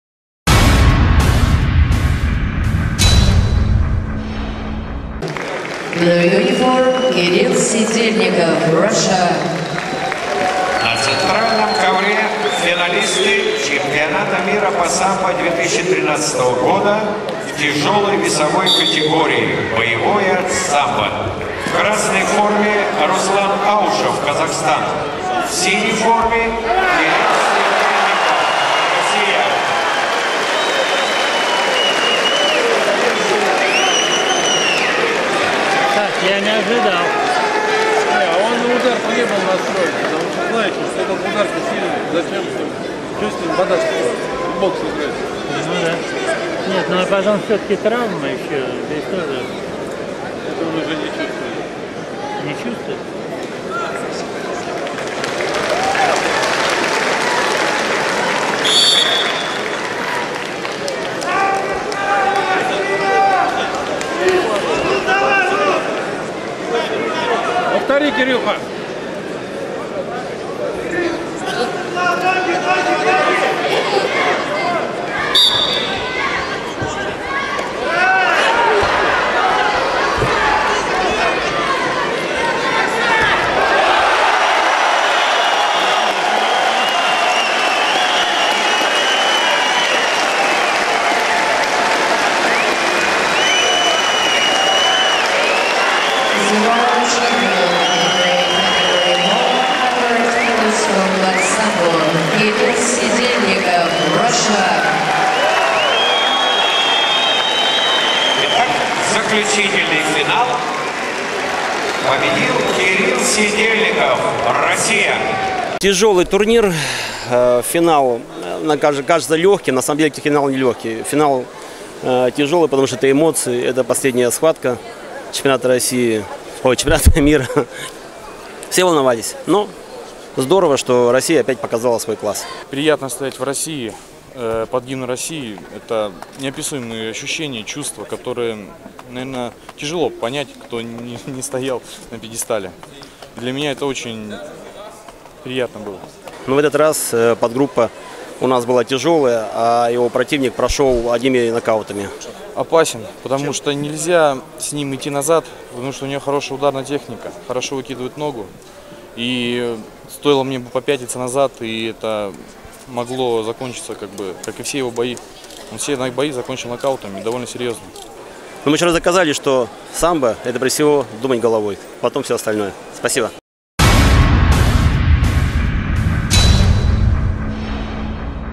Чемпионат мира по самбо 2013 \ финал \ Кирилл Сидельников vs Руслан Аушев
Финальная схватка на чемпионате мира по боевому самбо в Санкт-Петербурге
Интервью с Владимиром Вороновым